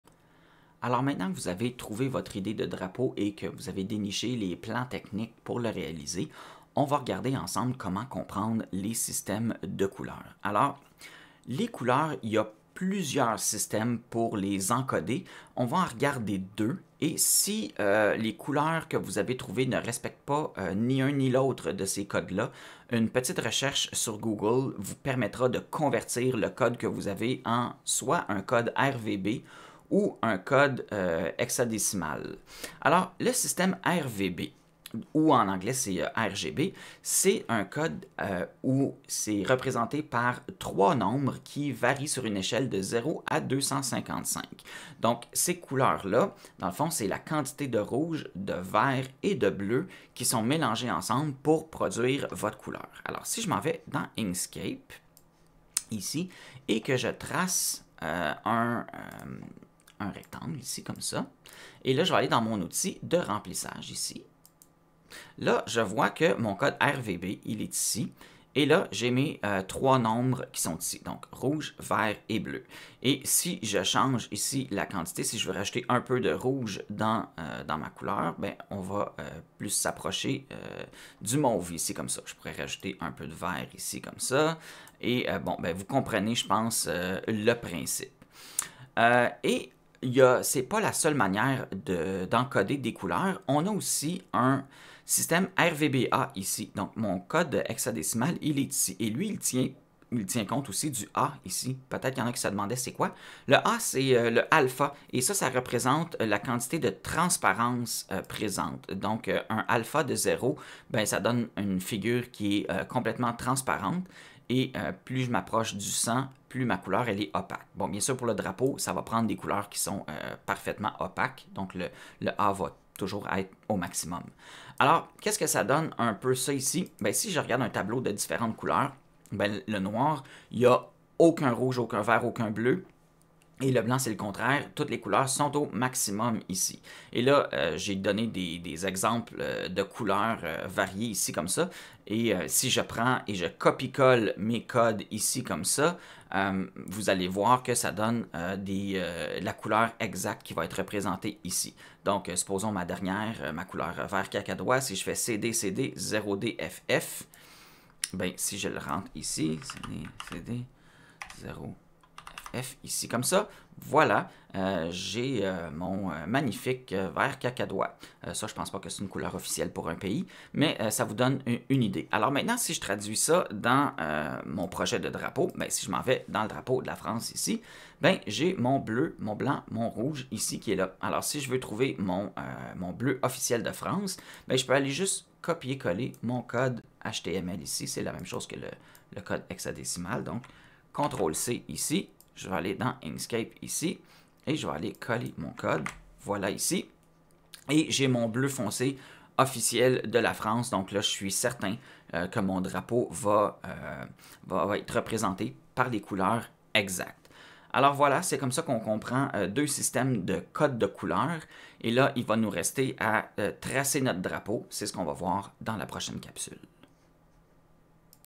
02 Drapeau Couleurs RVB et HEX
Comment comprendre les codes de couleur et les appliquer dans Inkscape